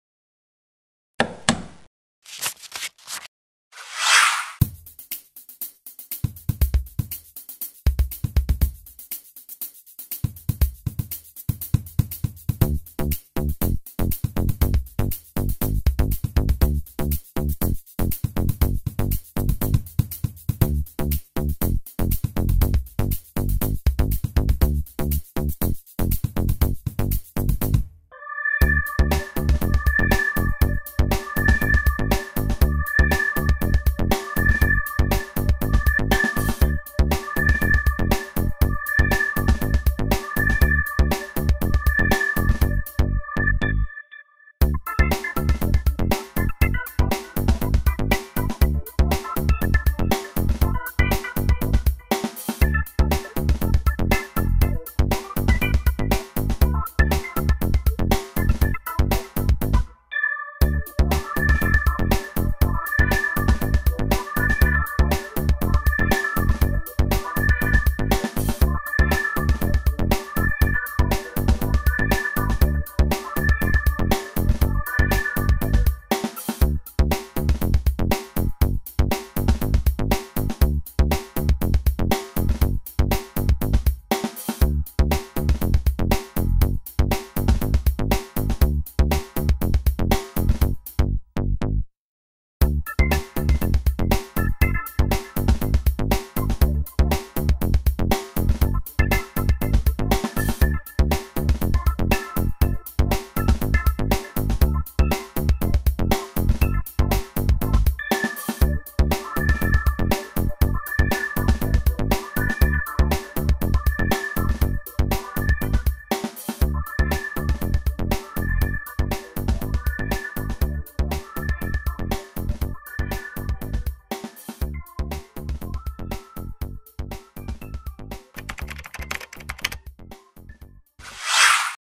Dell Venue Pro: Unboxing
This is an unboxing of the Dell Venue Pro.